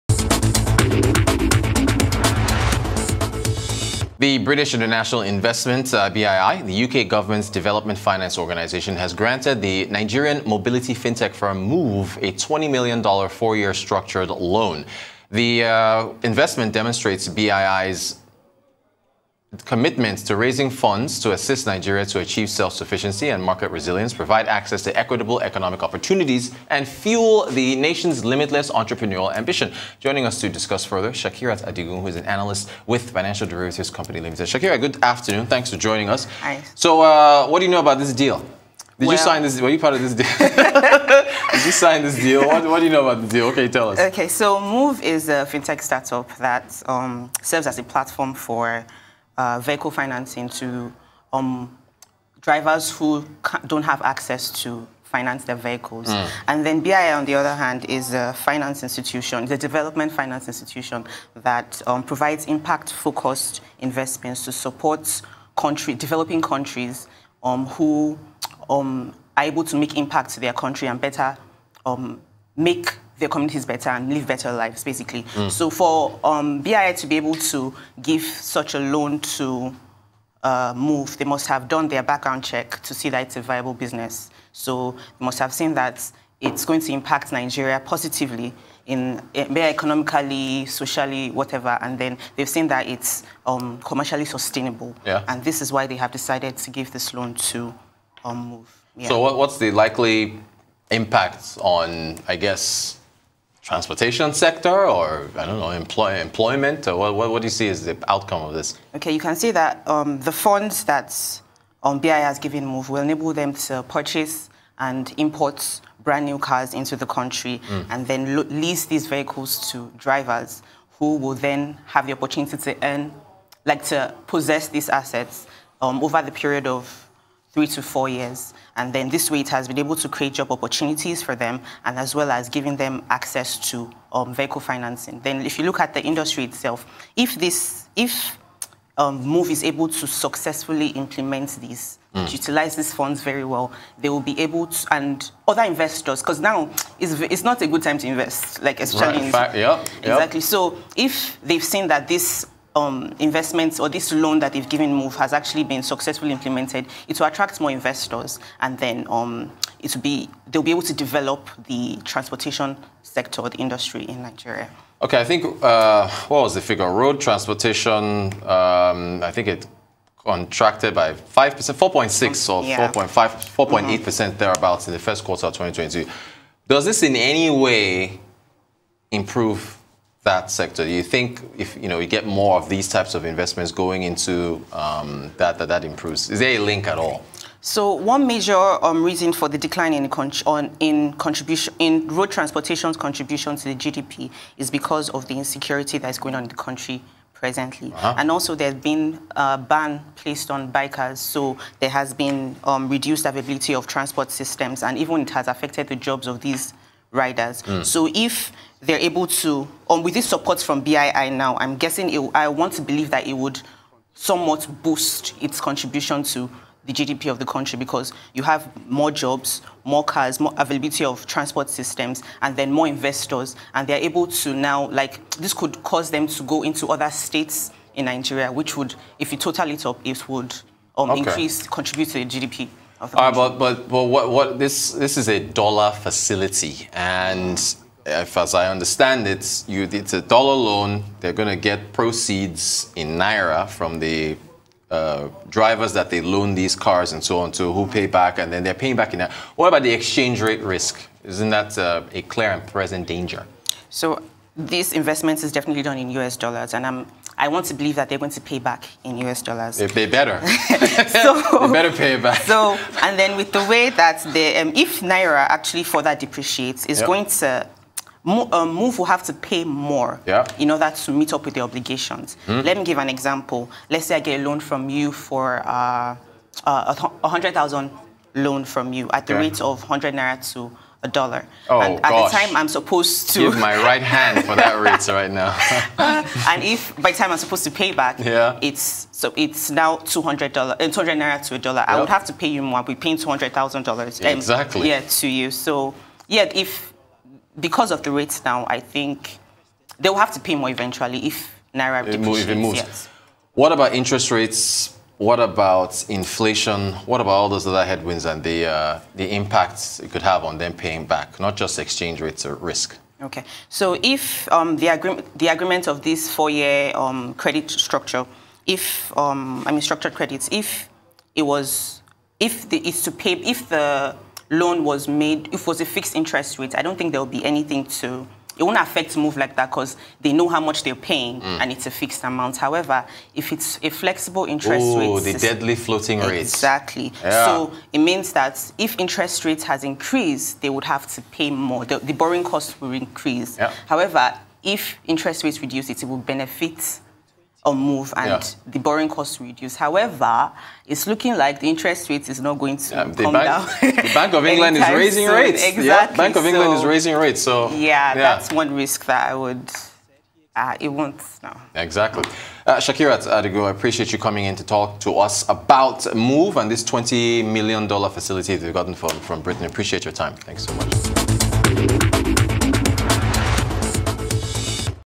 Road Transportation GDP Decline Down to Insecurity - Shakirat Adigun
Analyst, Financial Derivatives Company Limited, Shakirat Adigun reviews BII's  investment with Moove to the Nigerian transport sector